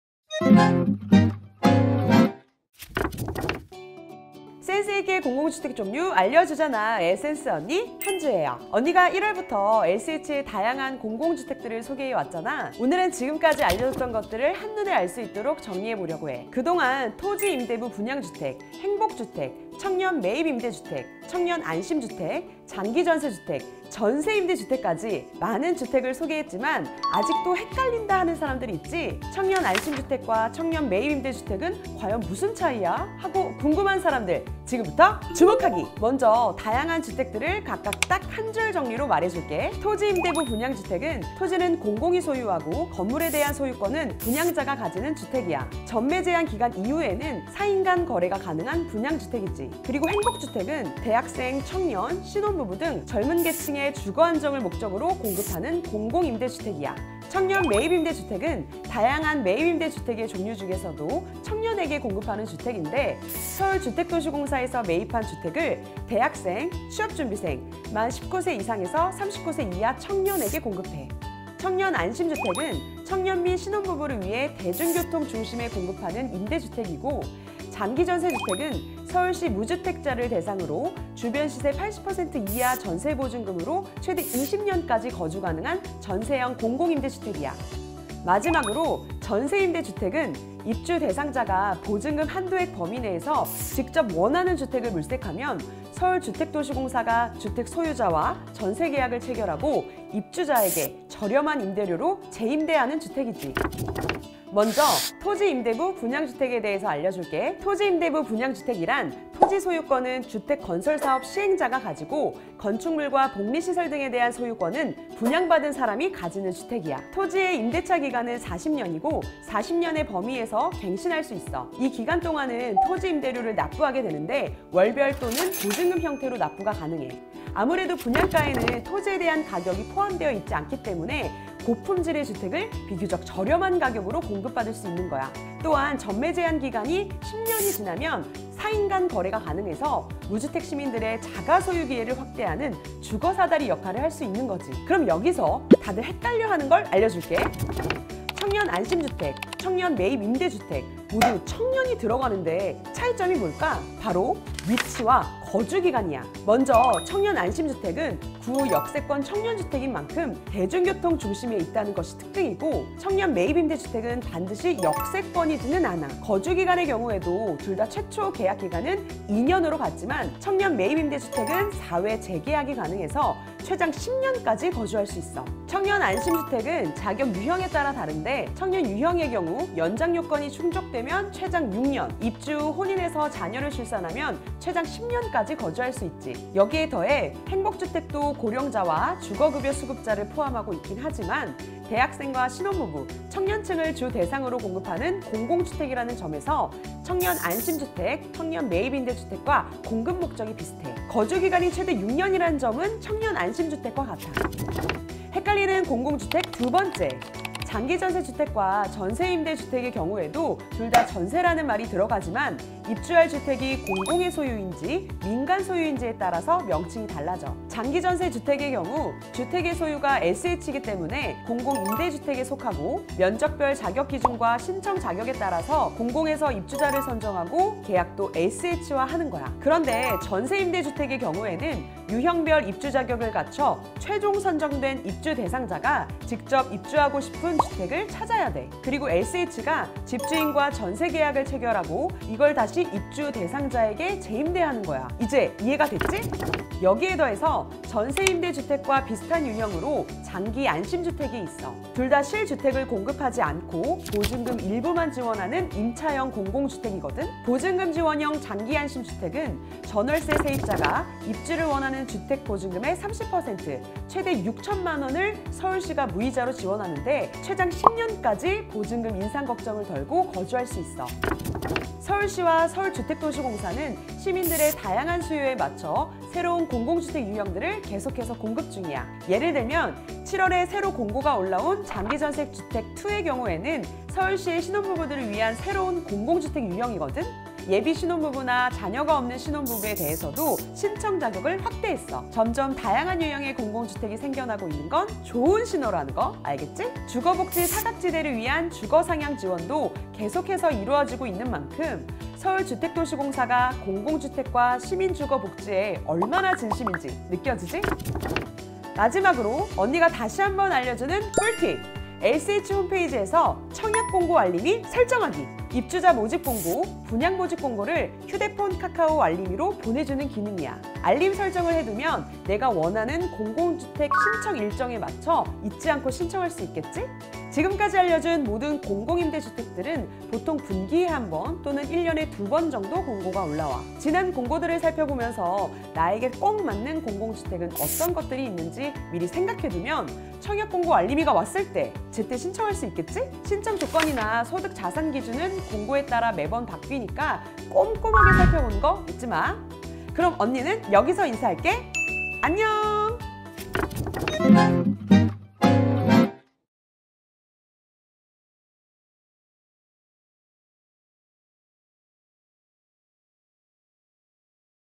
[센스있게 공공주택 종류 알려주잖아] 7편_ 총 정리 편
1월부터 SH의 다양한 공공주택들을 소개한 센스언니!
토지임대부 분양주택, 행복주택, 청년매입임대주택,
청년안심주택, 장기전세주택, 전세임대주택까지.

개념정리부터 비교분석까지 한눈에 알고 싶다면,
‘센스있게 공공주택 종류 알려주잖아’를 놓치지 마세요!